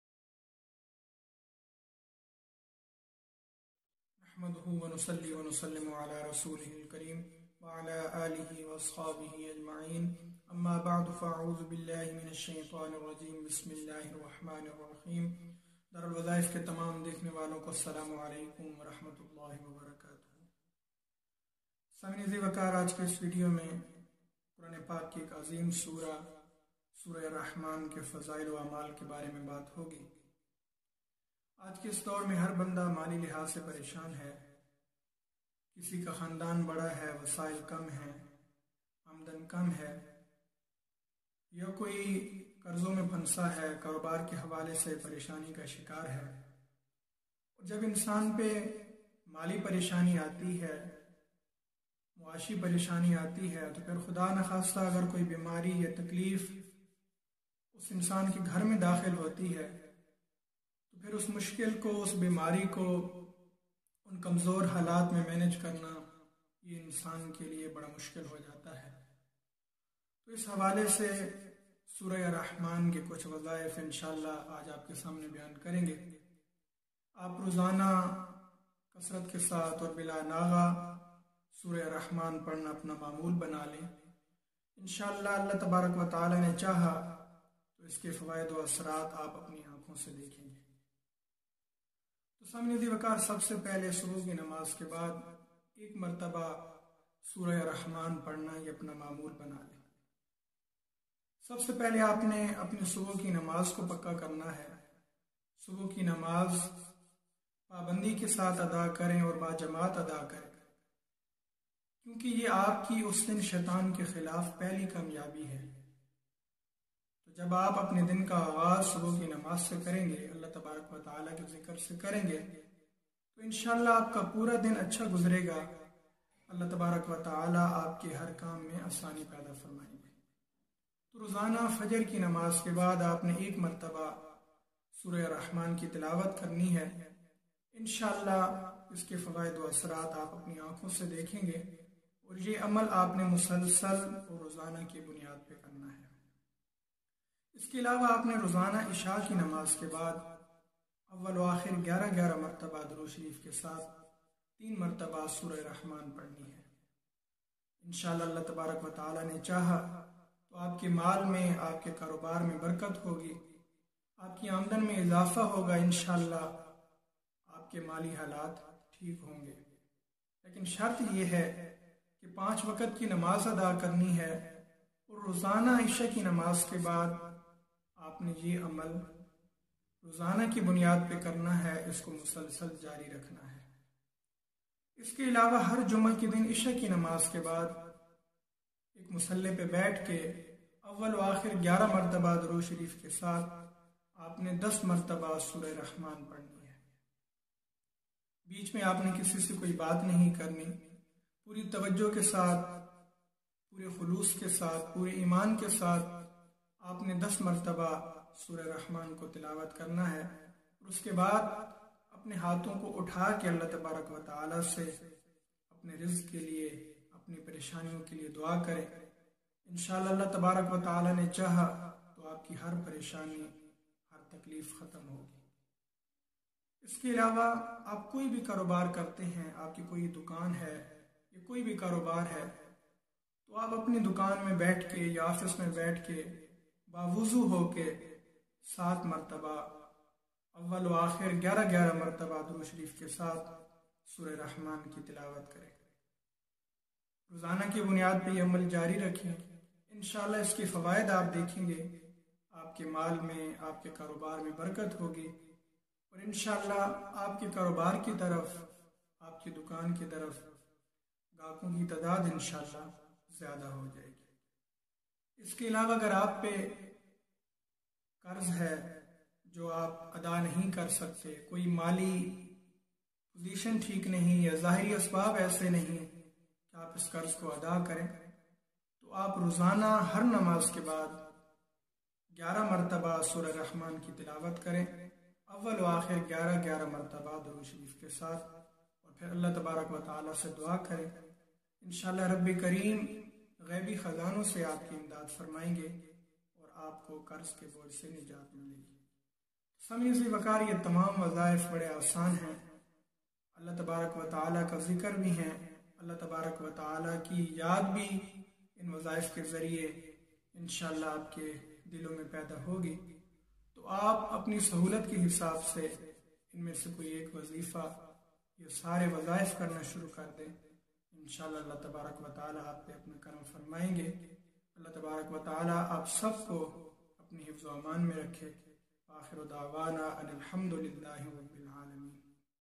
Juma k Din Isha Ki Namaz K Bad 11 Bar Surah Rahman Parhany Ki Fazliat | Rizq Mein Barkat Ka Wazifa
Dear Brothers And Sisters! Aj ki is video mein rizaq mein aur barkat kelay Surah AL Rahman kay bahot hi khas wazaif batay gay hain Jin Say In Sha Allah Rizaq mein itna izafa hoga k soch hai apki shart ye hai k daily Ye Surah Rahman parhain.

Juma K din Isha ki namaz k bad parhay say wo kuch milay ga jo apki soch hai. Surah Rahman k parhany qarz say nijat milay ge aur
karobar mein barkat hogi.

Allah Hum Sub ka Haami-o- Nasir ho!

My New Video:

Also Check More Videos Related Islamic Wazaif